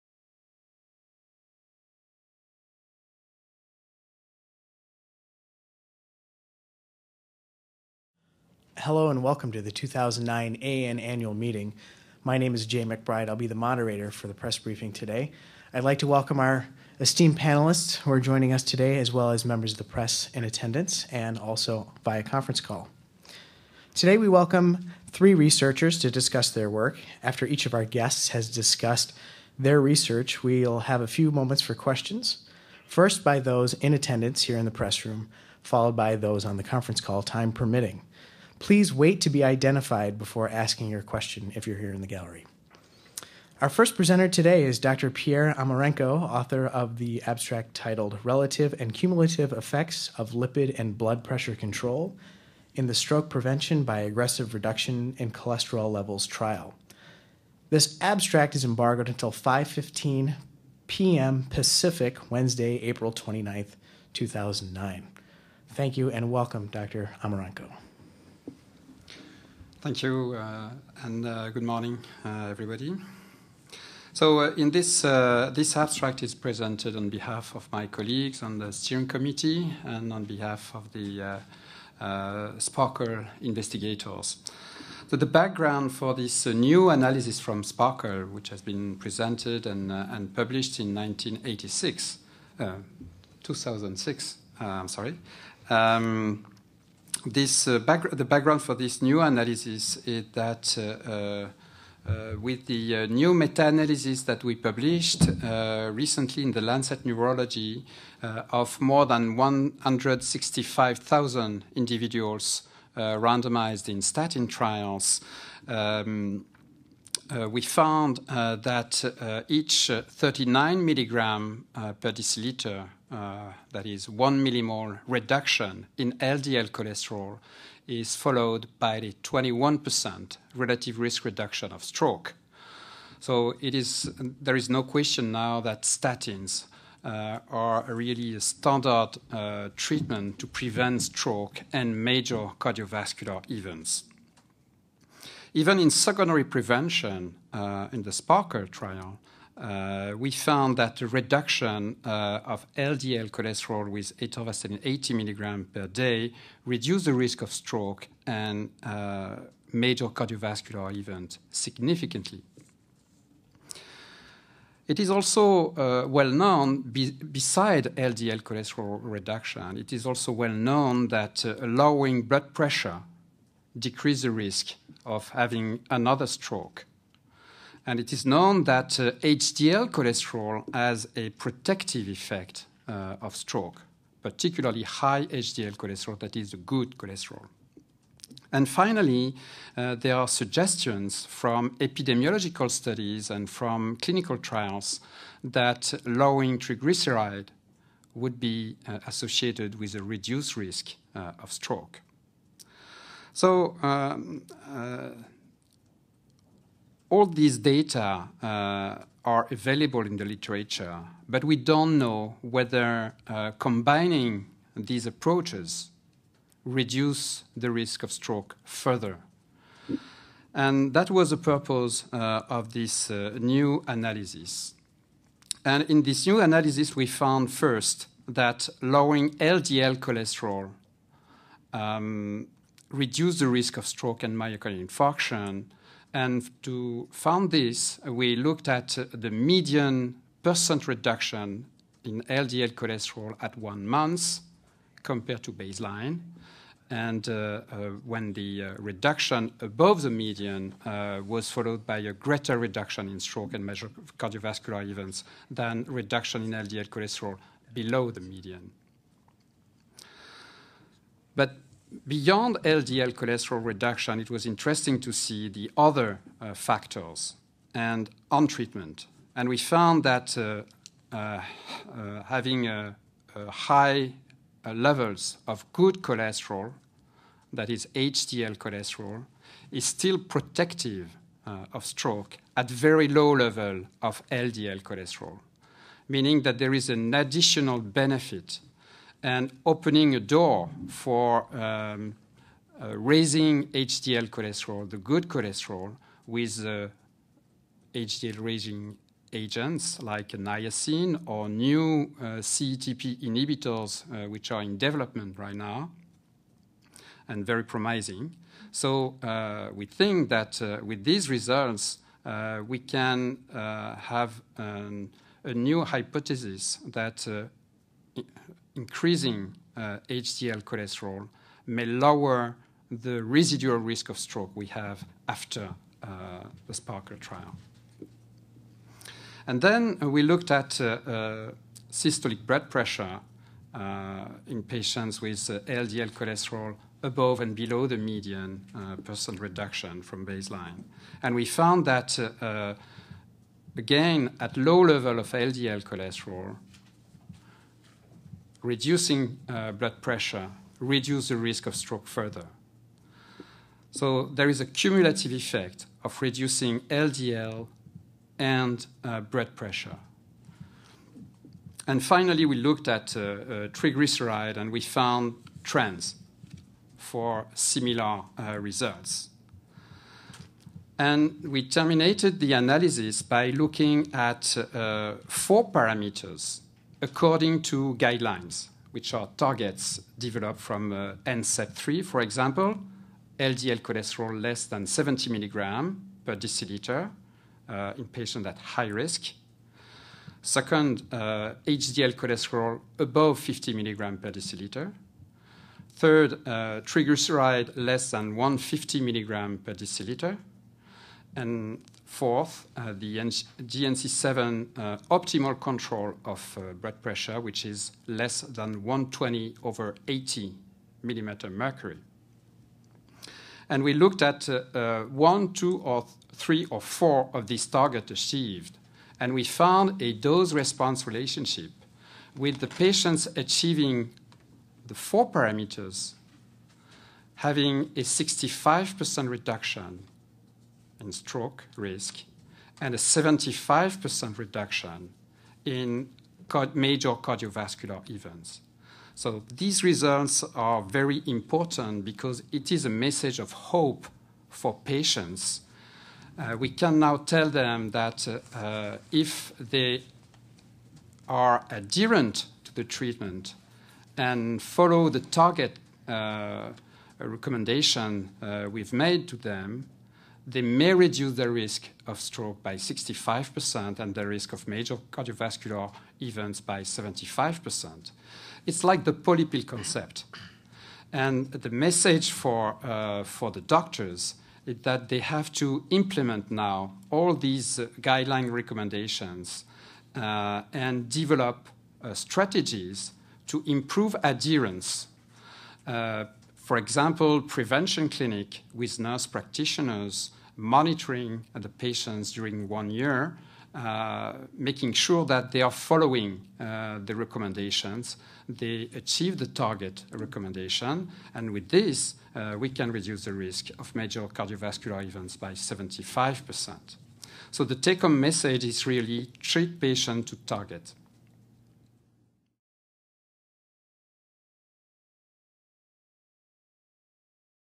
Controlling Cholesterol, Blood Pressure Adds Up to Prevent Stroke - American Academy of Neurology
Press Briefing: Pierre Amarenco, MD discusses his research on preventing future strokes by managing cholesterol and high blood pressure after a stroke, at the American Academy of Neurology's 61st Annual Meeting.